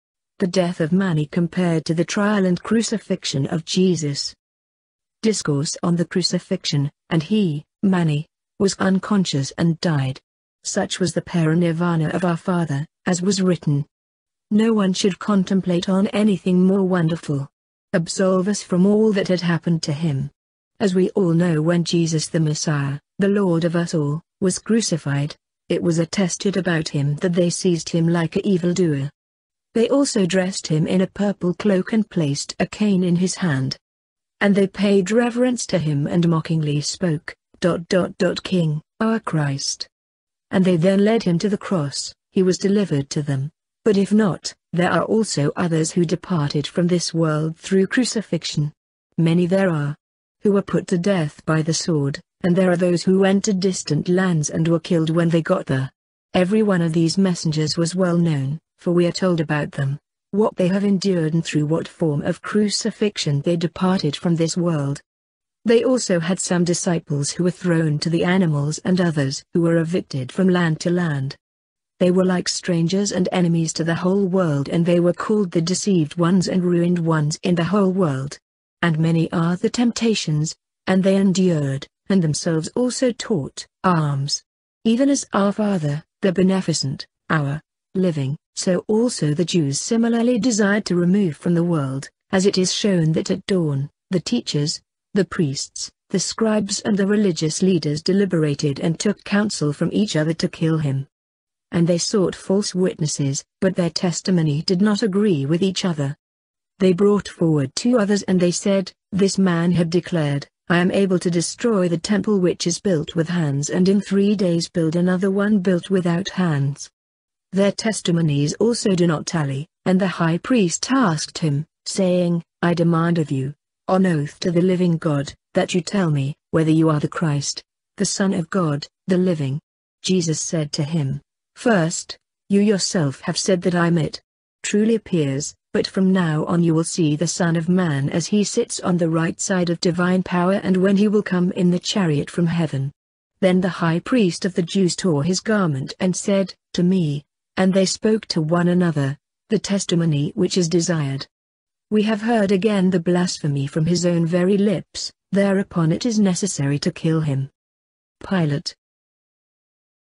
Manichaean Scripture: The Death Of Mani Compared To The Trial And Crucifixion Of Jesus
Religious scripture from the religion known as Manichaeism that is still extant in China.